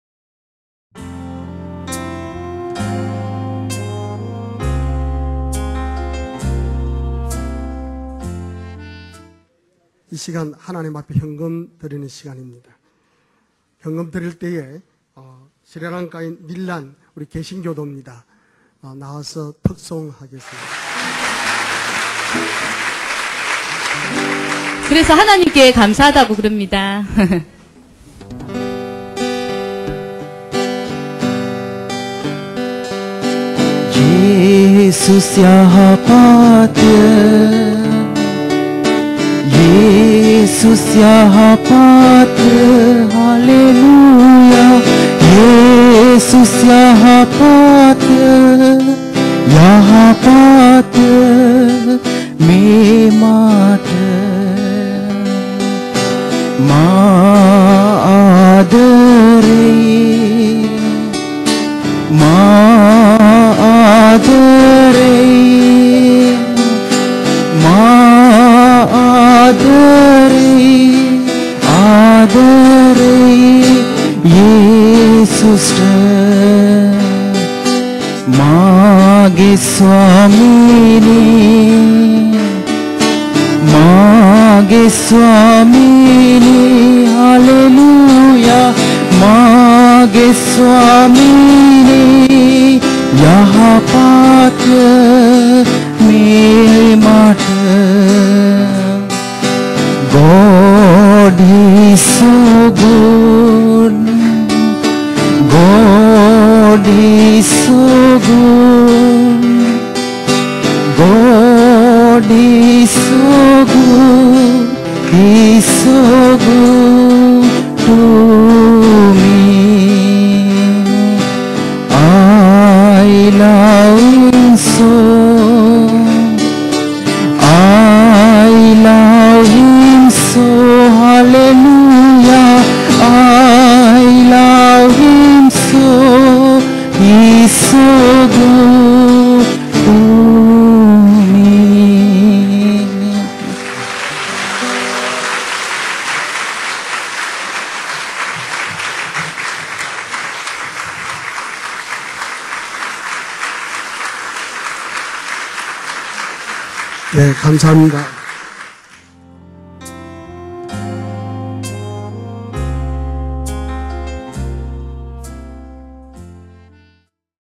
NILAN DE SILVA ;My First Song Bumin Church In Pusan South Korea
BUMIN CHURCH PUSAN SOUTH KOREA . Nilan de Silva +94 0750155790